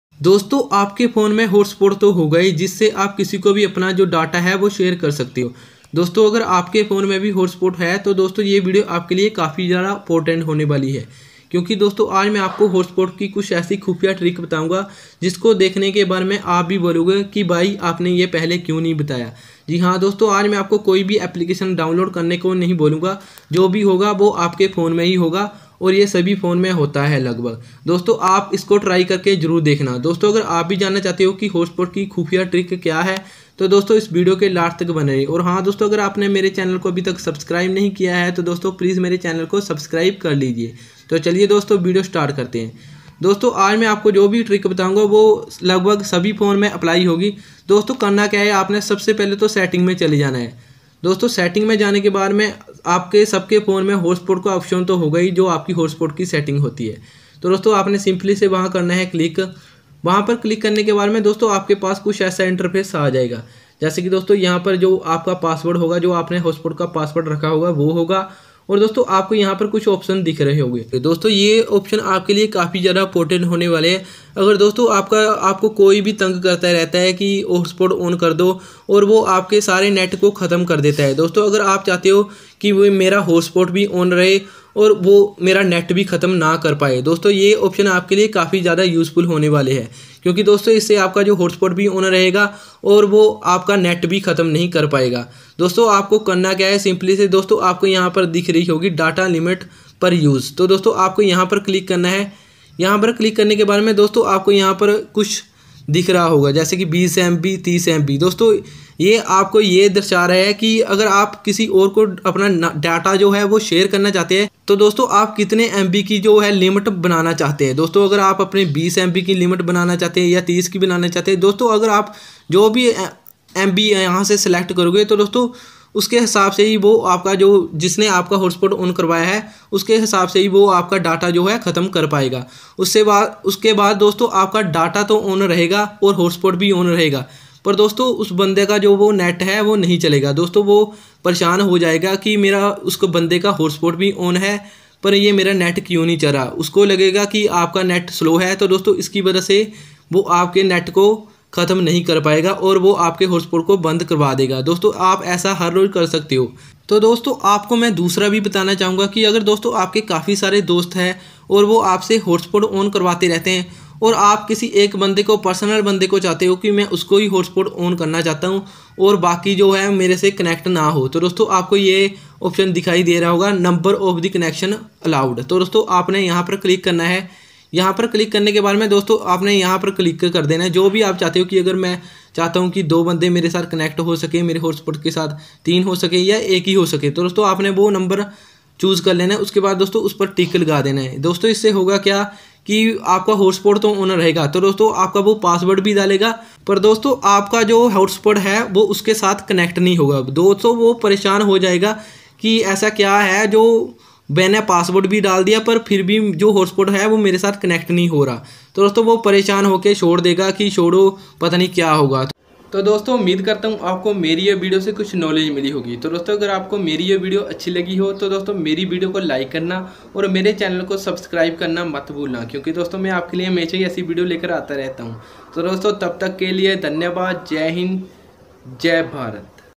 Hotspot secret setting in your mobile
Hlo dosto aaj main aapko is video main batau ga ki agar aapka koi hotspot bar bar on karvata rehta hai to aapko kya setting karni chahiye ki bo aapka net na khatam kar sake to dosto agar aap b jan na chahte ho hotspot ki khufiya trick to video ke last tak bane rahe